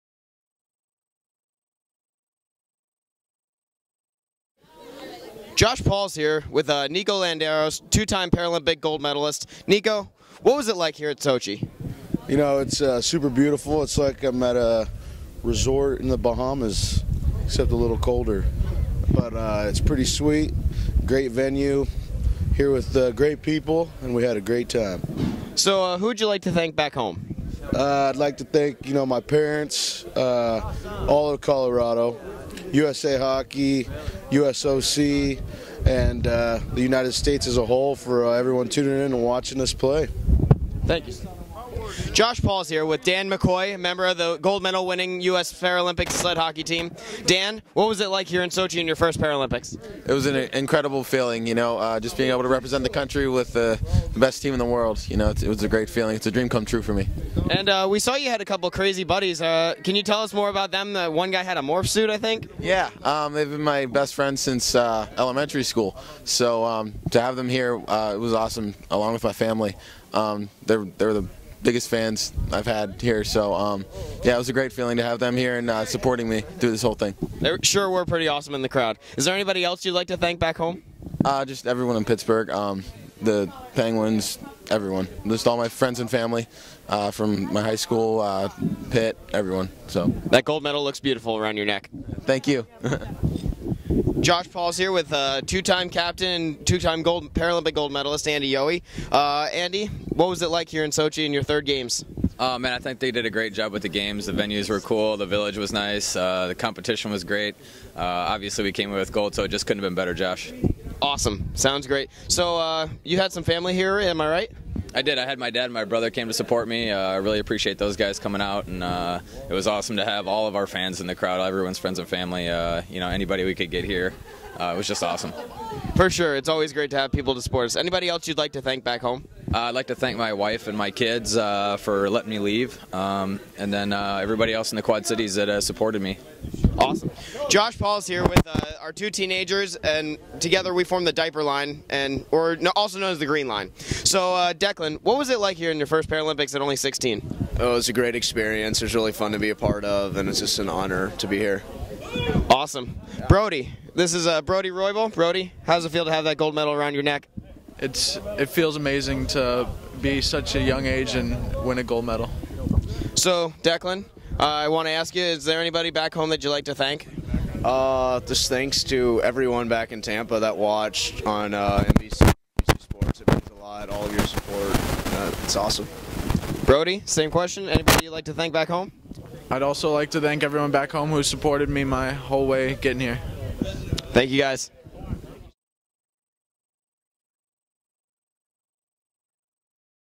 2014 Paralympics Closing Ceremony Thank You
Josh Pauls talks to his teammates before the 2014 Paralympic Winter Games Closing Ceremony about what they'll remember from the Games.